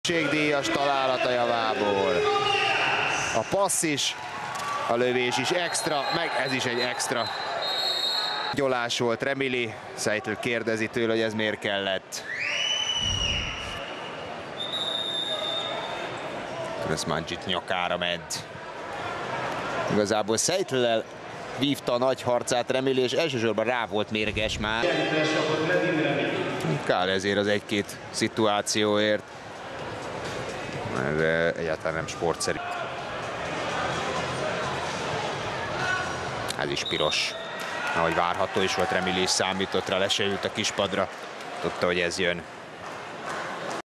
Red Card | Nedim Remili vs Mladen Krsmancic | Telekom Veszprem - Csurgoi
Handball Telekom Veszprem vs Csurgoi KK. K&H Liga Férfi Kézilabda 2023/24